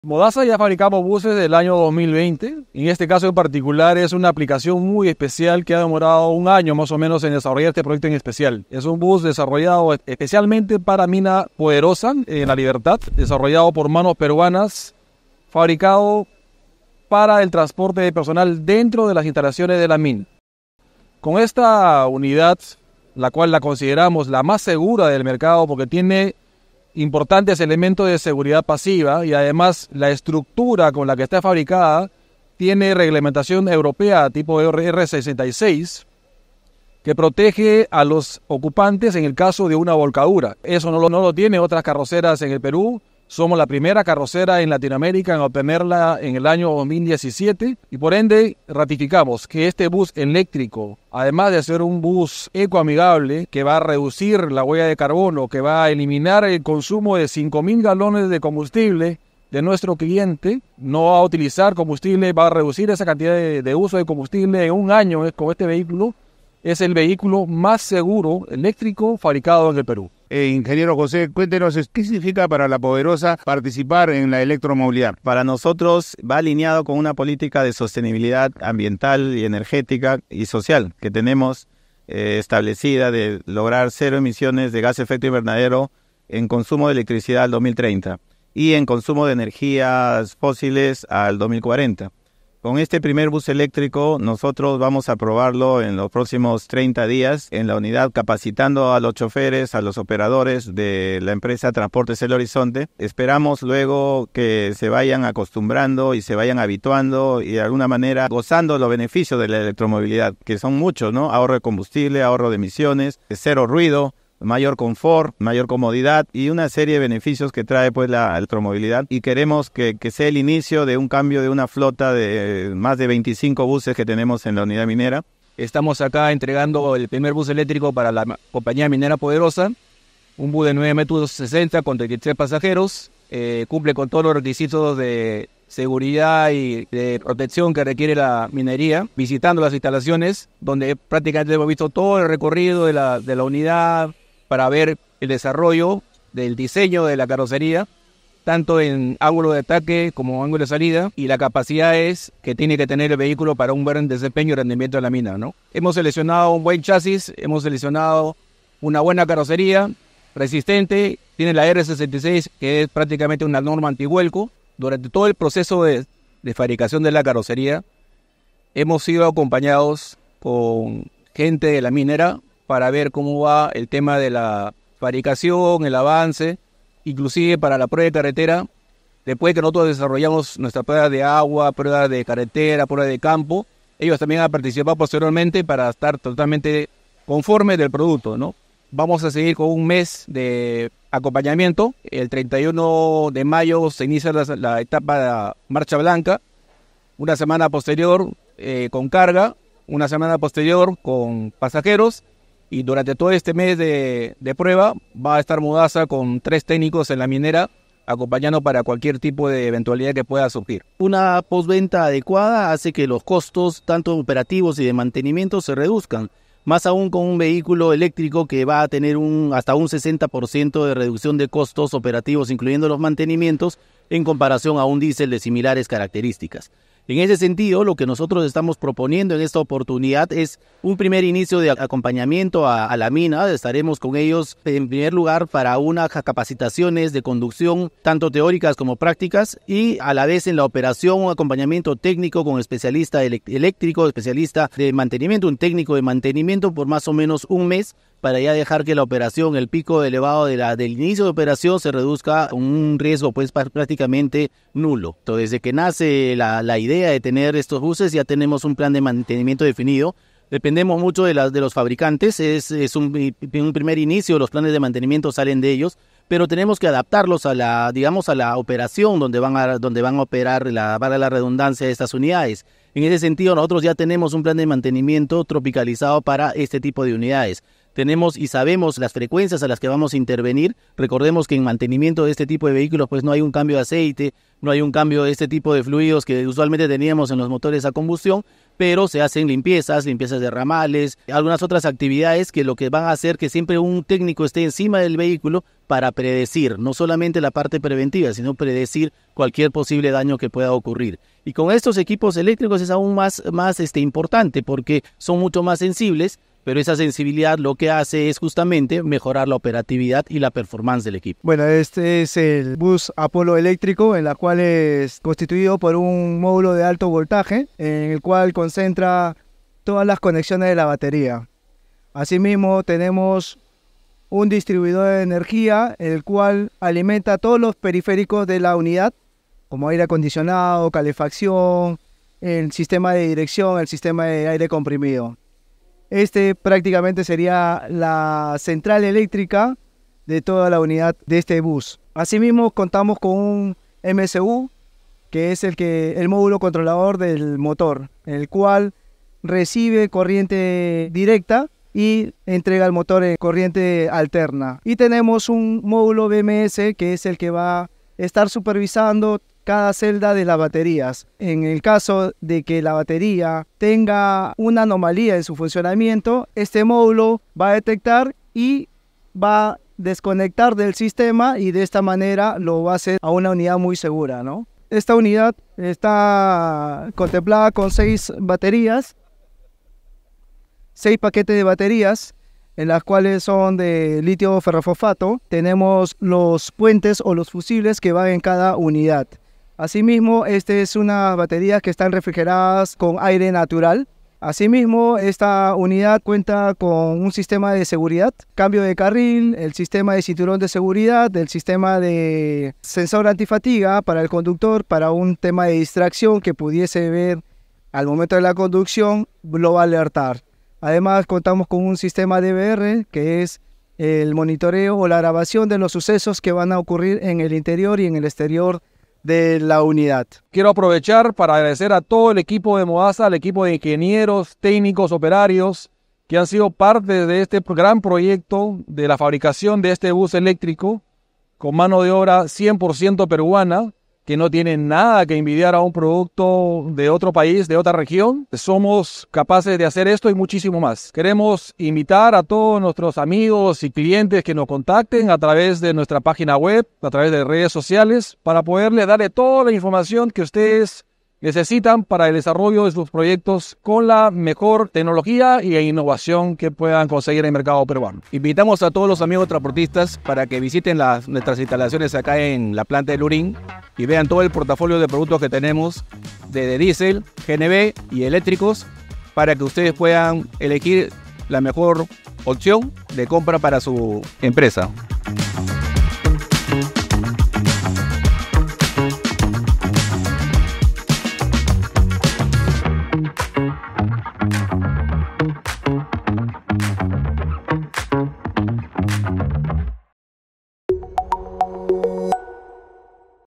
MODASA: BUS 100% ELÉCTRICO para la minería peruana | Made in Perú
Modasa, líder en la fabricación de soluciones de transporte y energía, se complace en anunciar la entrega de un bus eléctrico de última generación a la Compañía Minera Poderosa. Este vehículo eléctrico, fabricado en Perú ha sido diseñado específicamente para cumplir con las estrictas especificaciones que exigen las operaciones mineras para el transporte seguro y eficiente de su personal.

El bus eléctrico de Modasa no solo representa un avance significativo en la movilidad sostenible y en la capacidad de ingeniería peruana, sino que también incorpora los más altos estándares de seguridad y comodidad para los trabajadores de Poderosa.

El bus presenta una seguridad reforzada al contar con el certificado europeo R66.02, garantizando la integridad estructural del vehículo en caso de vuelco, además de sistemas avanzados que incluyen frenos ABS y cinturones de seguridad en todos los asientos.

Este vehículo es adaptable a terrenos mineros, gracias a su suspensión robusta, diseñada para manejar sobre las superficies más exigentes de las operaciones mineras, y neumáticos especializados, resistentes al desgaste y a los daños en terrenos irregulares.

En términos de eficiencia y sostenibilidad, su propulsión eléctrica contribuye a conseguir cero emisiones, promoviendo la sostenibilidad ambiental, uno de los pilares de Modasa. Esta configuración de vehículo posee una autonomía de 200 km, de recorrido al 100% de carga.